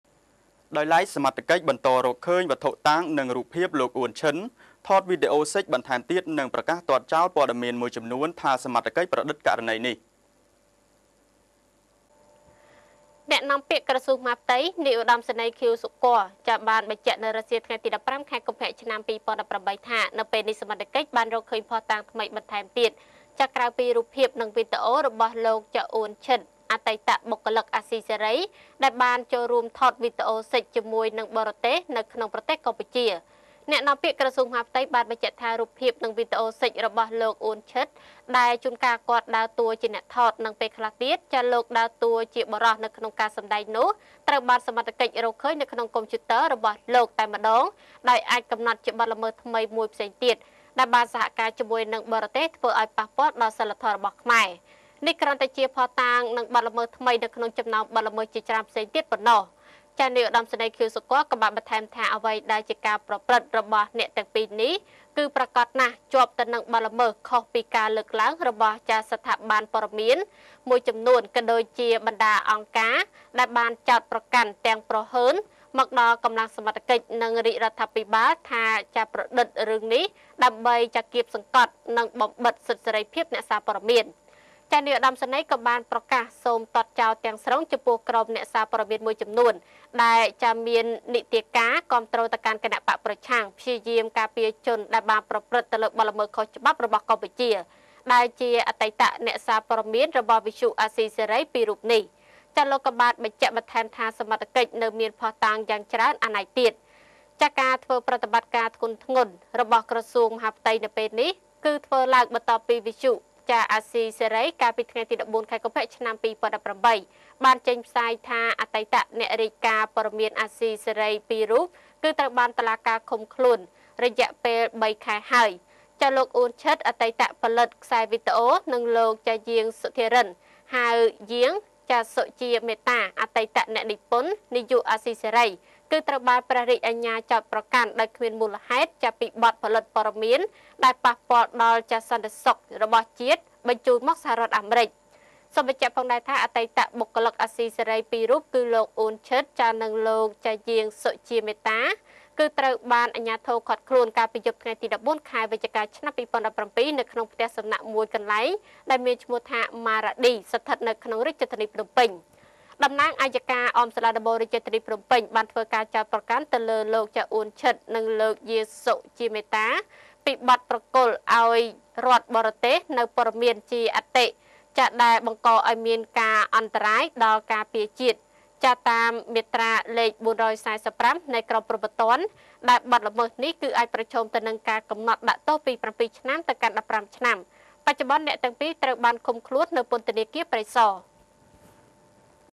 សមត្ថកិច្ចបន្តរកឃើញភស្តុតាងនិងរូបភាពលោក អ៊ួន ឈិន ថត
សមត្ថកិច្ចបន្តរកឃើញភស្តុតាងនិងរូបភាពលោក អ៊ួន ឈិន ថតវីដេអូសិចបន្ថែមទៀត និងប្រកាសទាត់ចោលព័ត៌មានមួយចំនួនថាសមត្ថកិច្ចប្រឌិតករណីនេះ